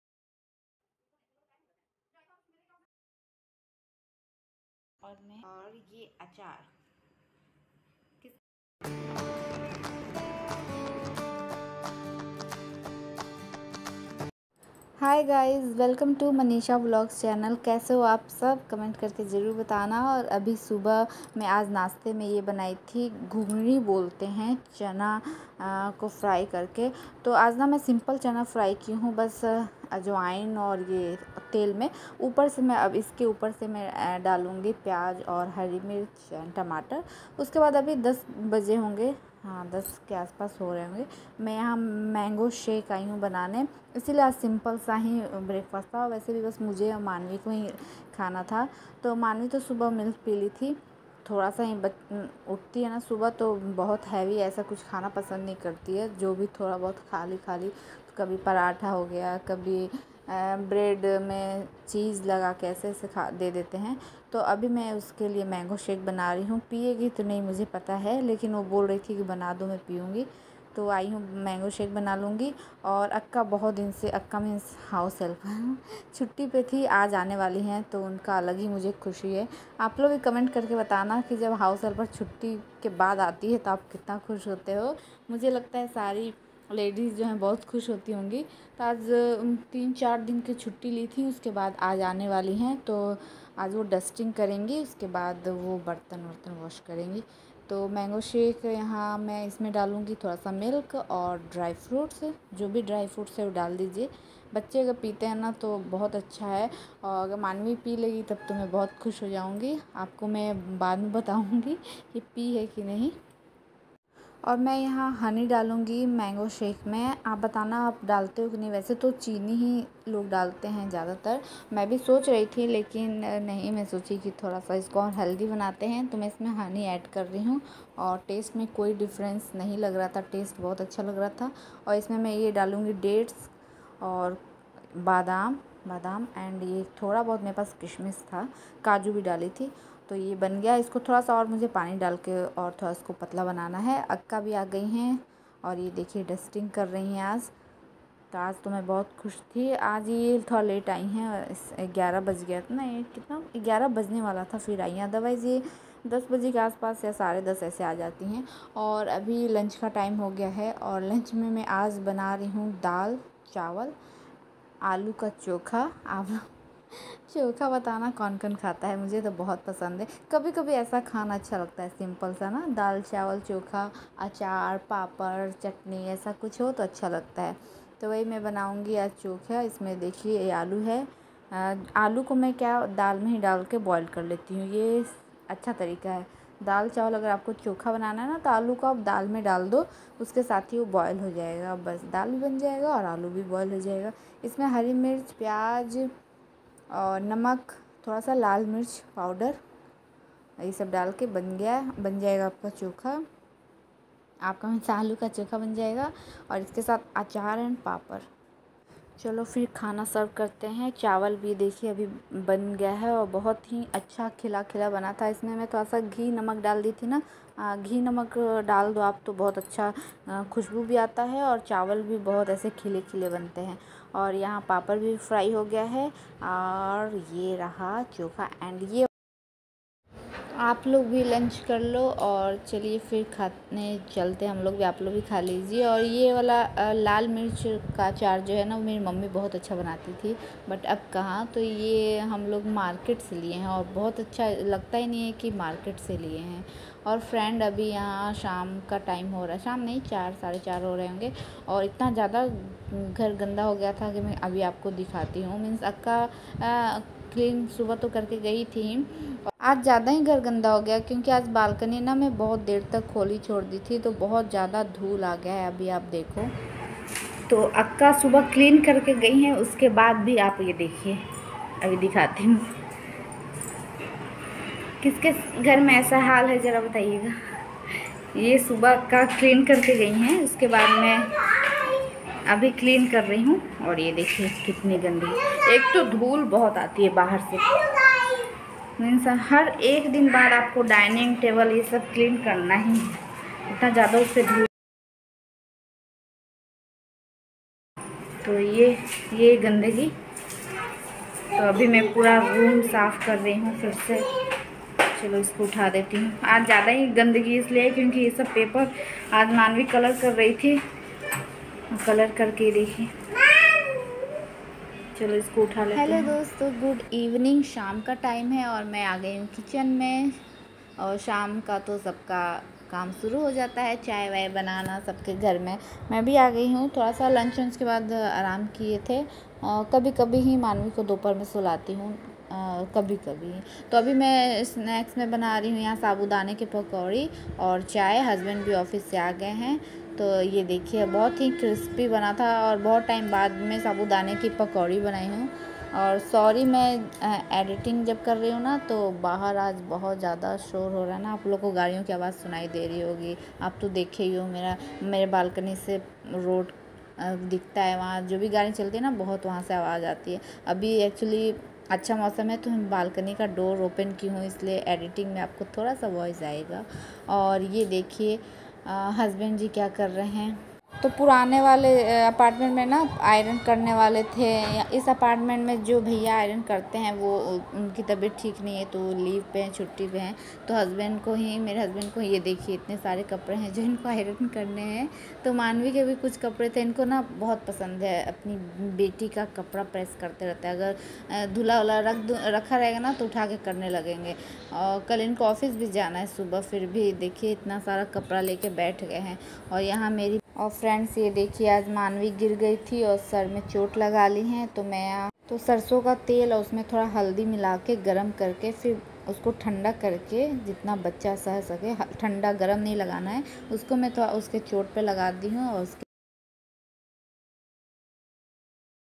दाल, चावल, चोखा Desi Khana🍛❤️| Baar Baar Aisa Kyu 😢| Garmiyo में कैसे कटता मेरा दिन 🙂
Daal,Chawal Chokha Desi Khana🍛| Fir se Chot Lg Gai🙁| Ye Kaam Krne Me Bhut Khushi Hoti H🙂
cleaning,lunch,
हाउस मेकर 🙋 डेली वलोग 🌹
day in my life
Manisha vlogs channel
videos
Indian youtuber vlog
family vlogs
mango shake 🍹
VLOG🌹